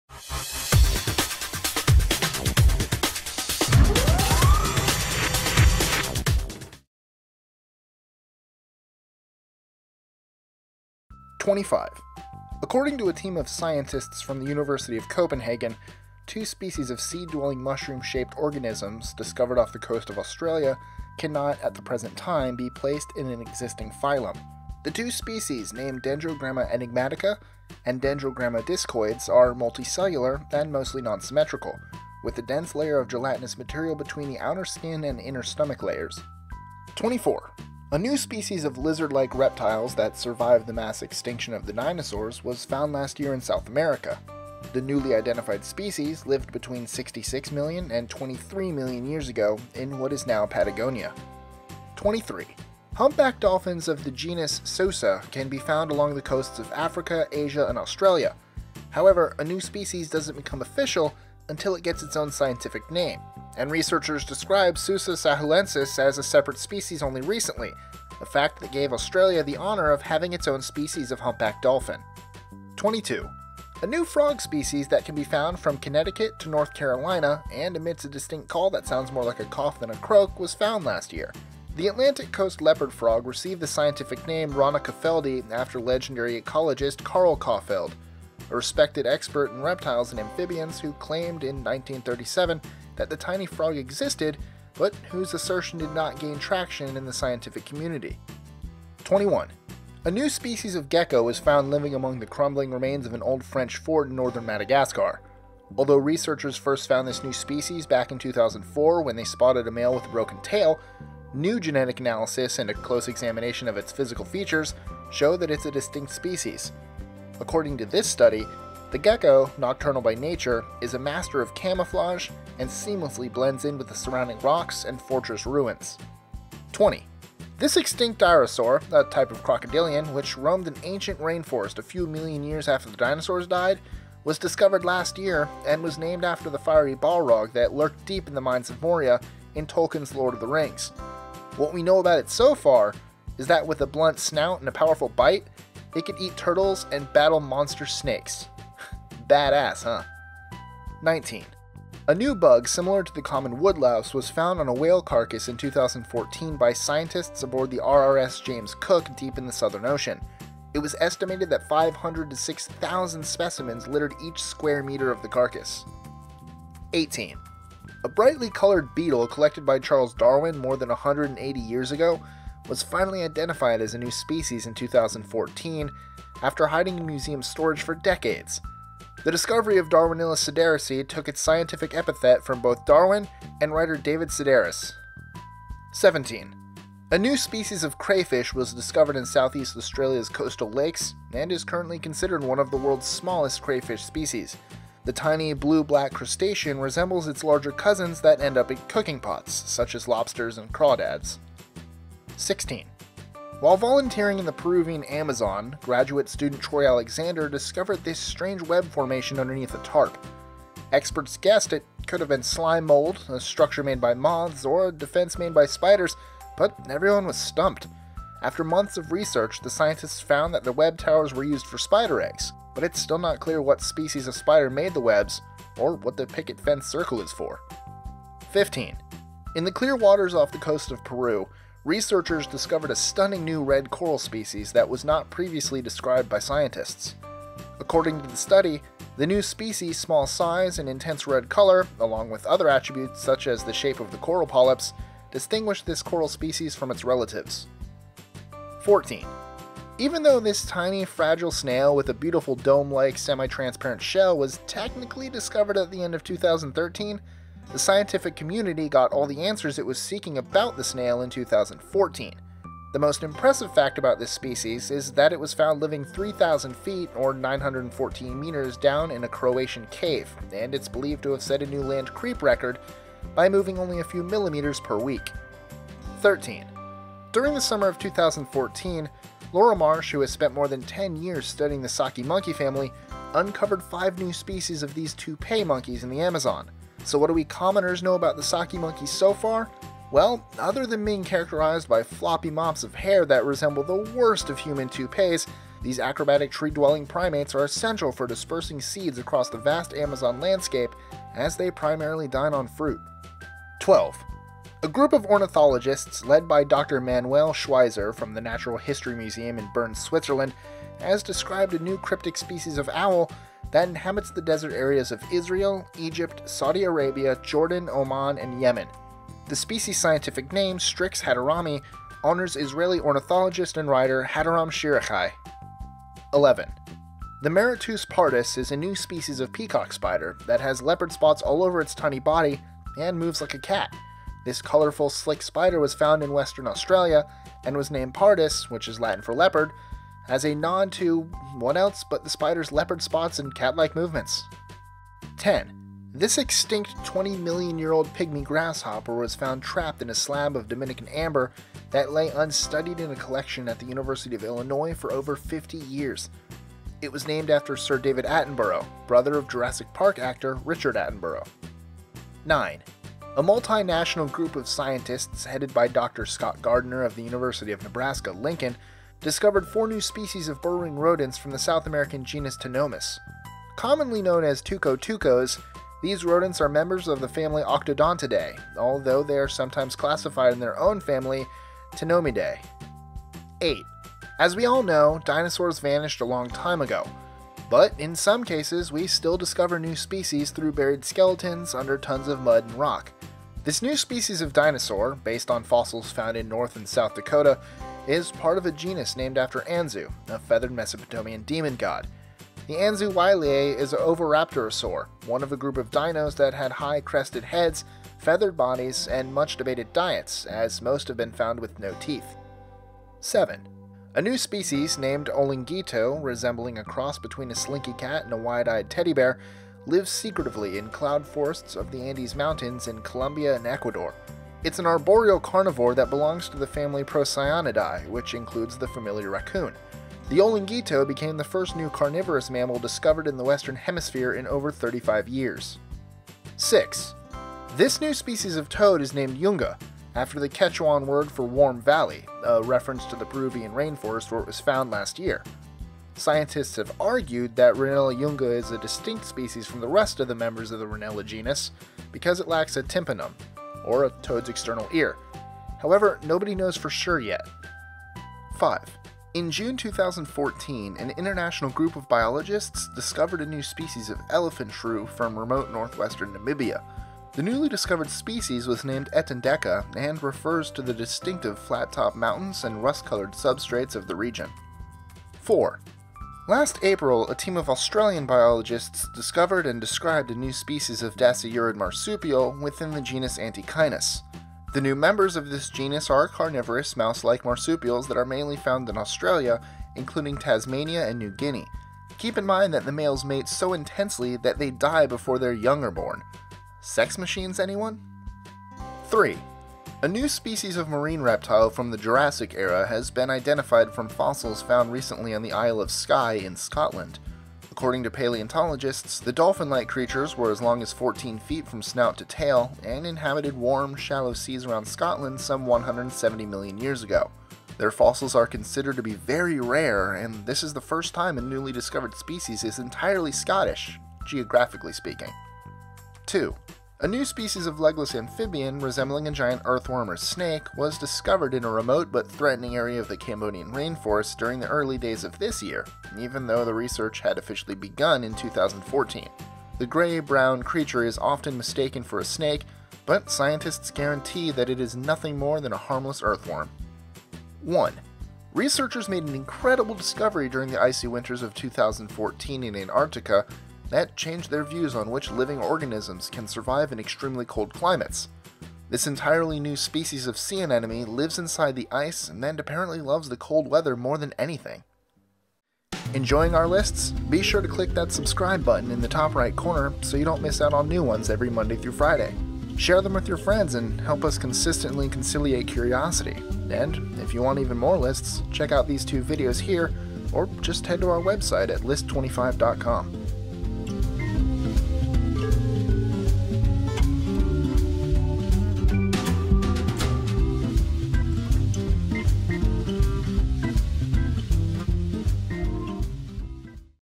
25 Incredible New Species Discovered In 2014
According to the scientific community more than 2 million species of plants, animals, and microbes on Earth have been identified, but most experts believe and estimate that there are many millions more waiting to be discovered. Although it will definitely take centuries to discover all these new species, we conducted a humble search and came up with 25 incredible new species discovered in 2014. To be clear, this list involves both living and non-living discoveries.

Preview:

Dendrogramma enigmatica & Dendrogramma discoides
Kawasphenodon peligrensis
Sousa sahulensis
The Atlantic Coast leopard frog
Paroedura hordiesi
Anthracosuchus balrogus
Bug species that lives on whale carcass
Darwinilus sedarisi
Gramastacus lacus
The Mysterious Structure
Fiery-Red Coral Species
Zospeum tholussum
Isabel’s Saki
The Desert Tawny Owl
Maratus pardus
Electrotettix attenboroughi
Tuco-tuco
Anzu wyliei
Olinguito
Rhinella yunga
Etendeka round-eared Sengi
Black-tailed Antechinus
Dearcmhara shawcrossi
Ichthyophis cardamomensis
The Anemone that Lives in Antarctica’s Ice